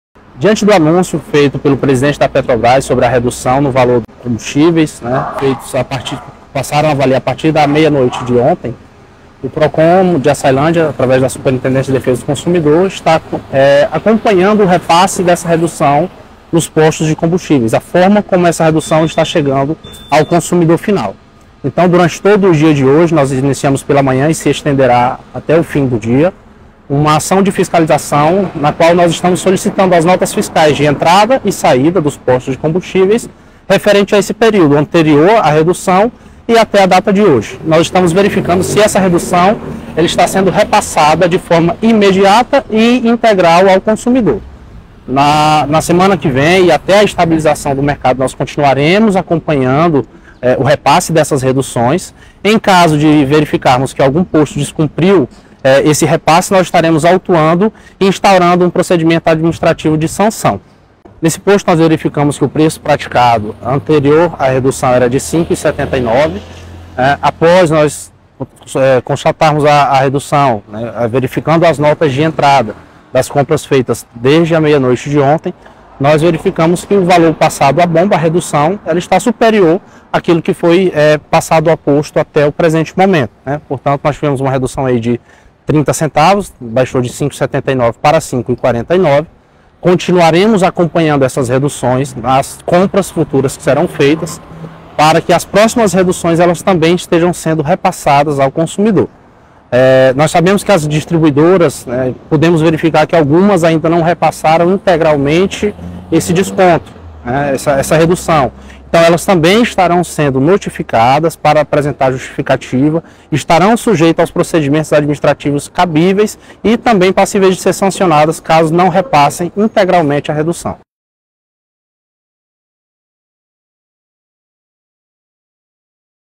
PROCON FISCALIZA E NOTIFICA POSTOS
O PROCON esteve fiscalizando e notificando postos e distribuidoras de combustíveis, no município de Açailândia, visando garantir o correto repasse da redução de valores ao consumidor final, conforme anúncio realizado pela Petrobras.

Durante a fiscalização os estabelecimentos devem apresentar as notas fiscais de entrada e comprovar o repasse imediato e integral ao valor cobrado na bomba. O não repasse da redução caracteriza prática infrativa, conforme disposto no Decreto Federal nº 2.181/97 e art. 39 da Lei 8.087/90 - CDC.

As ações de fiscalização se estenderão ao longo das próximas semanas e, caso encontre alguma irregularidade praticada, formalize denúncia junto ao PROCON com fotos e localização do posto em questão, ajudando a direcionar o trabalho da equipe de fiscalização.

Prefeitura de Açailândia - Compromisso com você